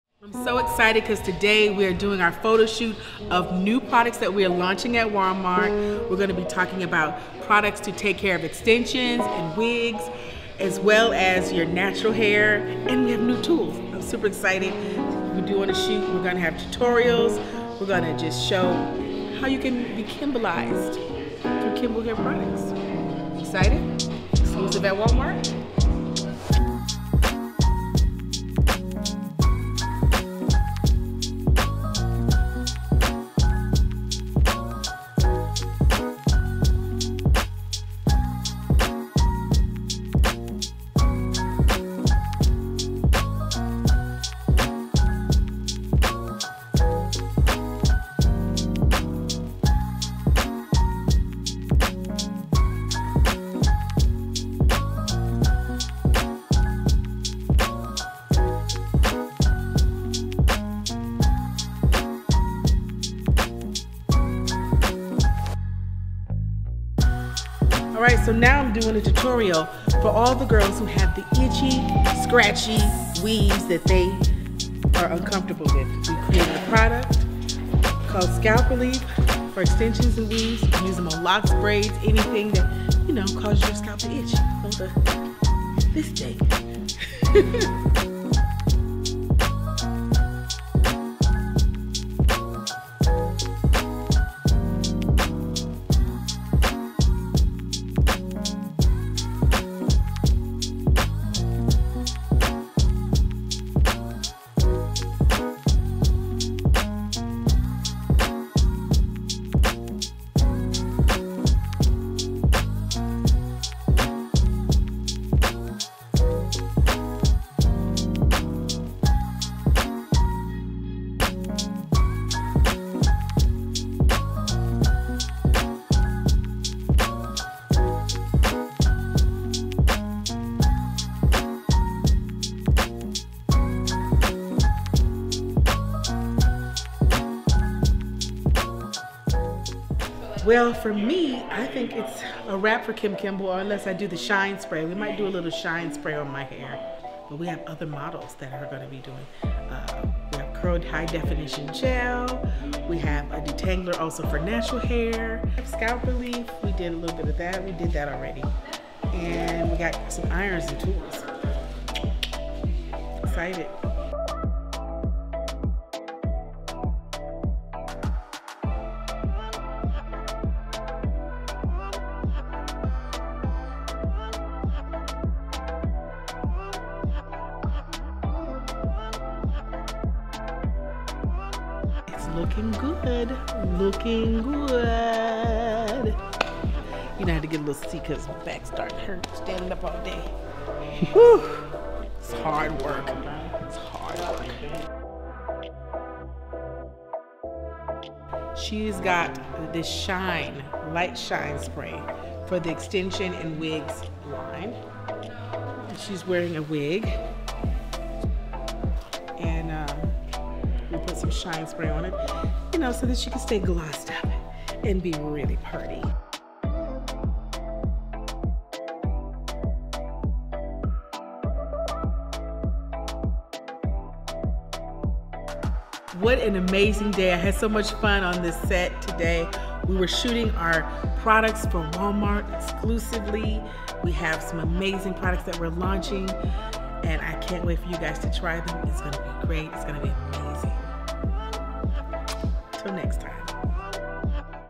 Kim Kimble Vlog | BTS my New Product Launch
Hi Everyone! I'm so excited to launch new products for you this year! I've added some things that you can now find at Walmart, including a Wigs and Extension care line. Come with me behind the scenes to the shoot for my new products. Enjoy ❤️

New Product Launches available at Walmart:

Perfect Leave in Detangler
Backstage Styling Serum
High Def Curl Gel

Wigs and Weave Care:
Instant Detangler
Light Shine Spray
Scalp Relief

New Tools
2 in 1 Iron
Vapor Infused Flat Iron